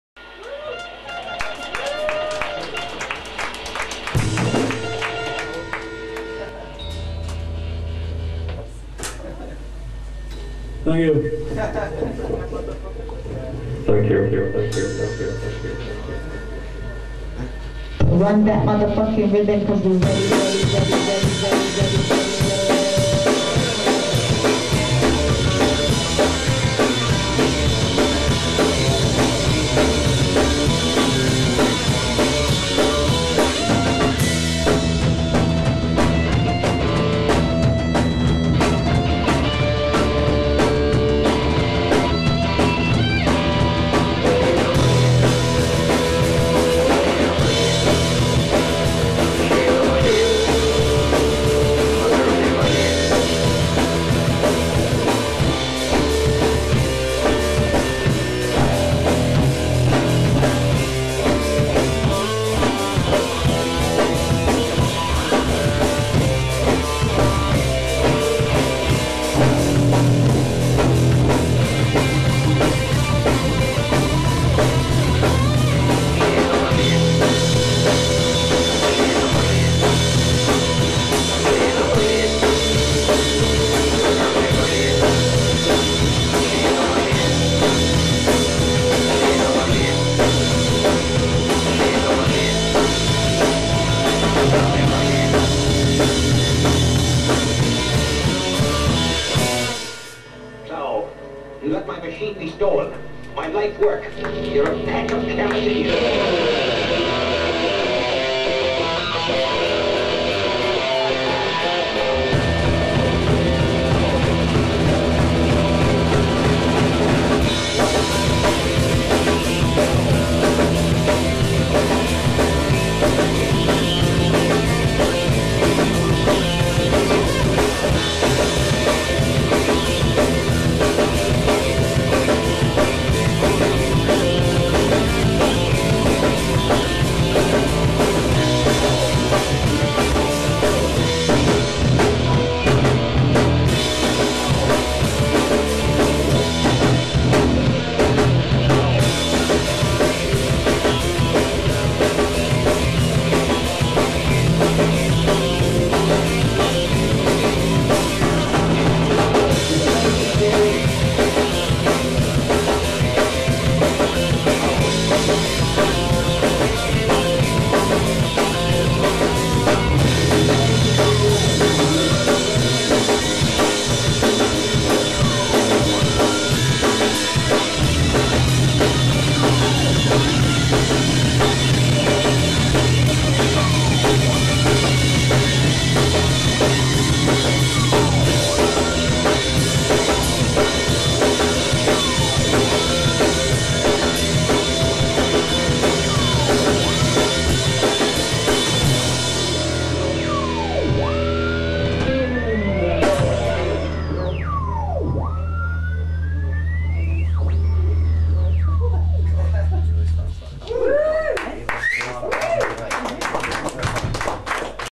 the bubble project (the band) live
2 sci-fi surf originals by the bubble project